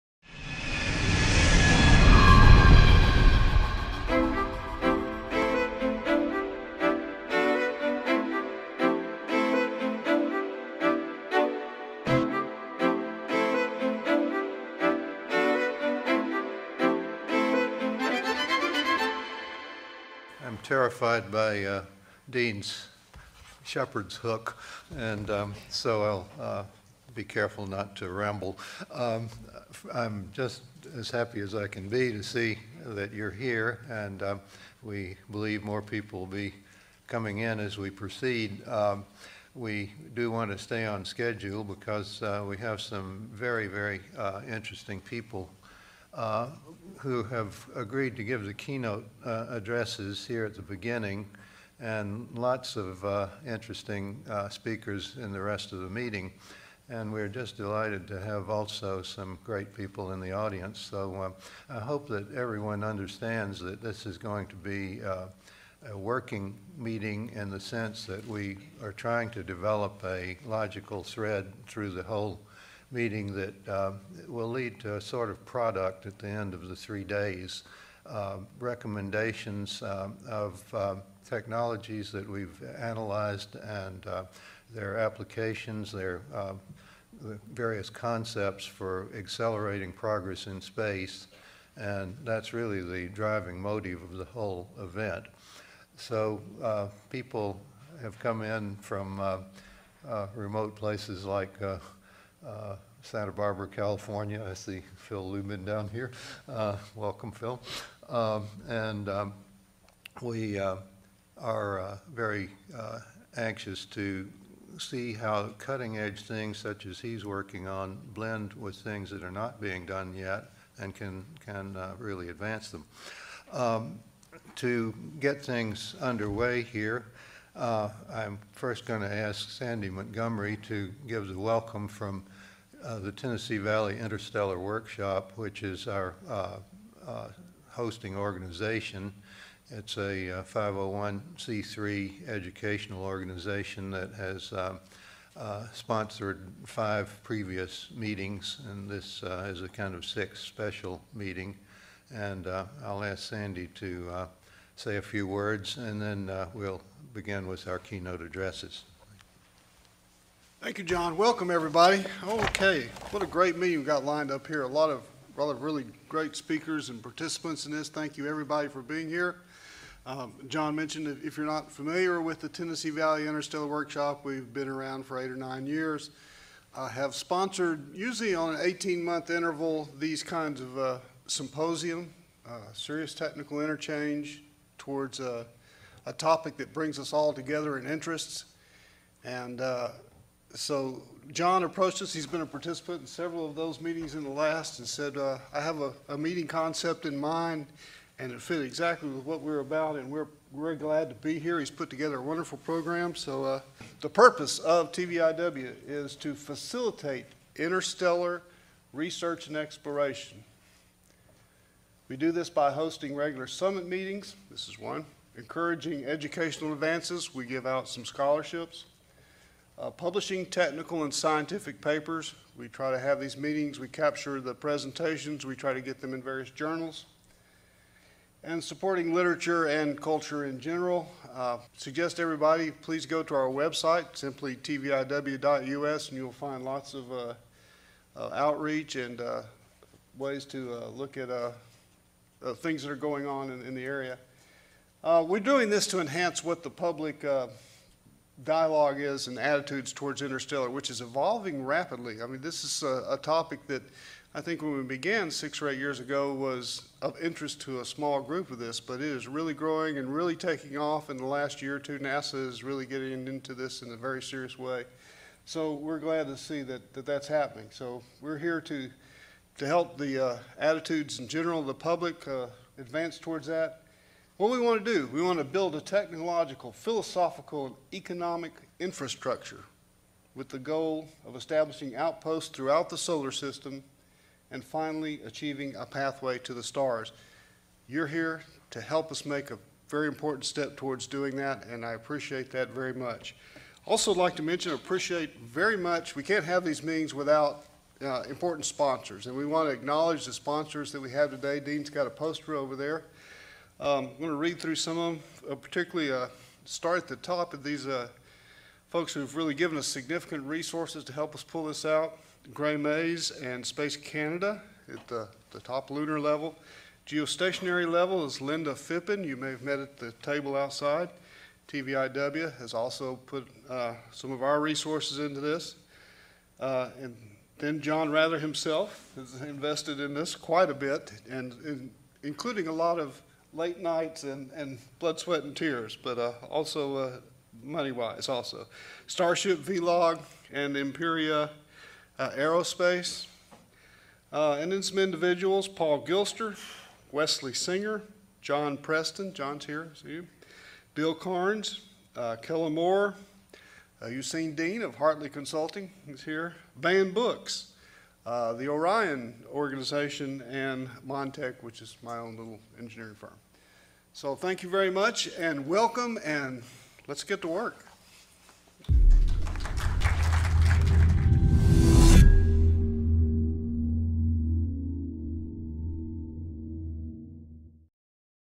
0-1 Dr. John D. G. Rather, Symposium Welcome and 0-2 Edward Montgomery, TVIW Welcome
Dr. John Rather and Edward "Sandy" Montgomery, President of TVIW, welcome participants to "The Power of Synergy" Space Symposium in Oak Ridge, TN at New Hope Center on Oct. 23-25, 2018.